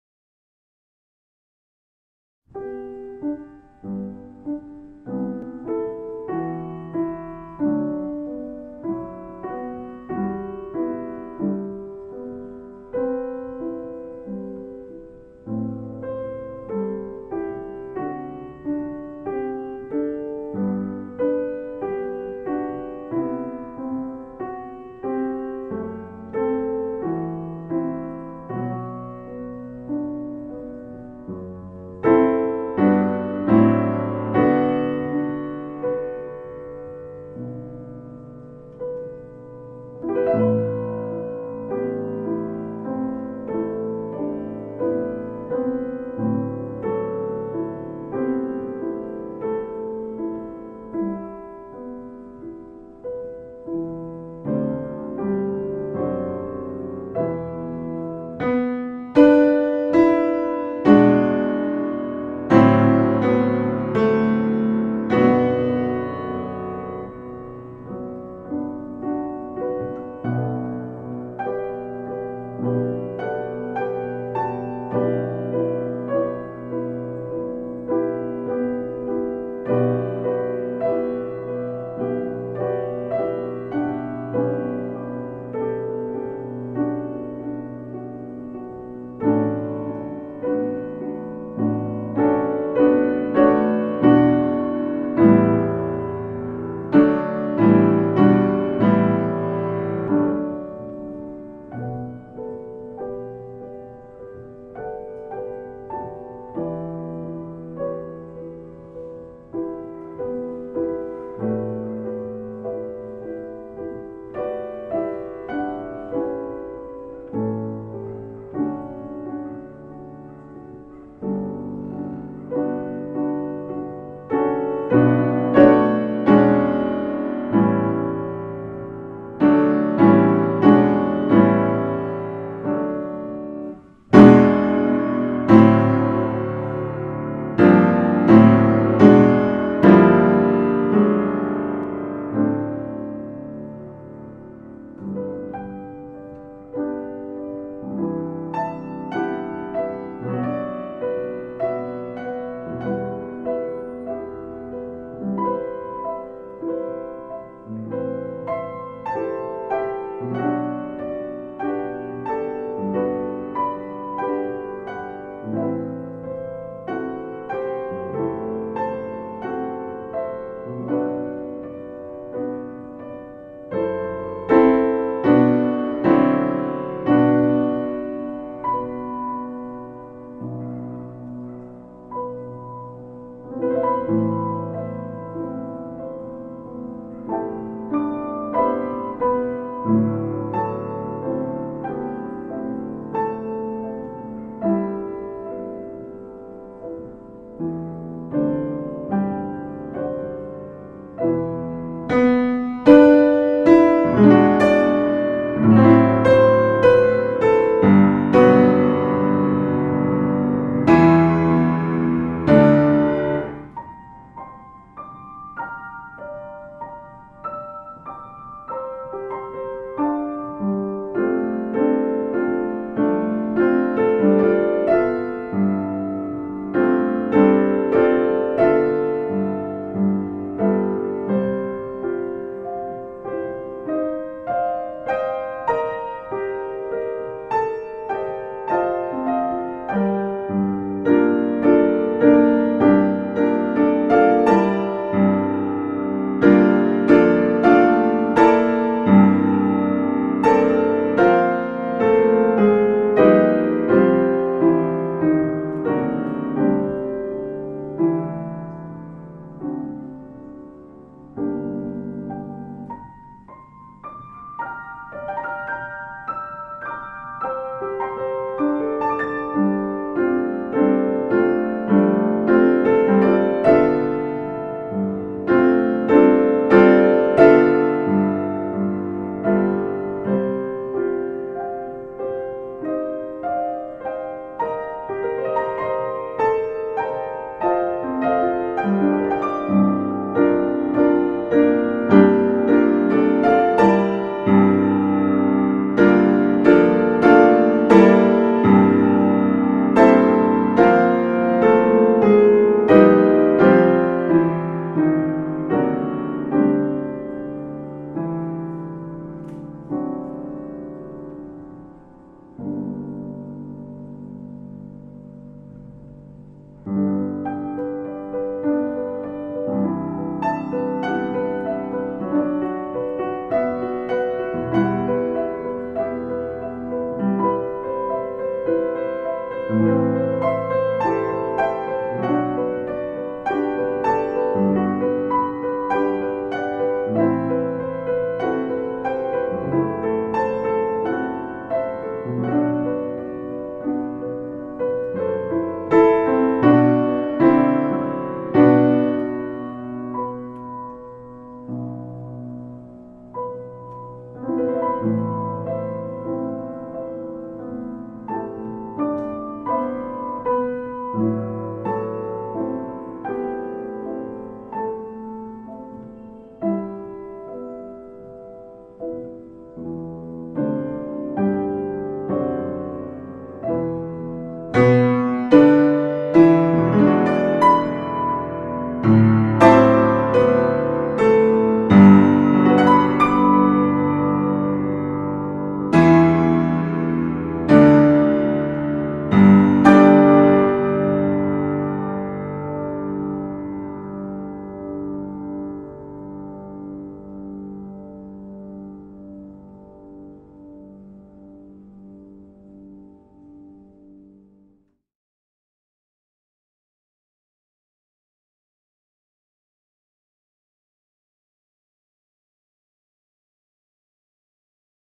O/W-2
A seven minute video featuring the music of Maurice Ravel.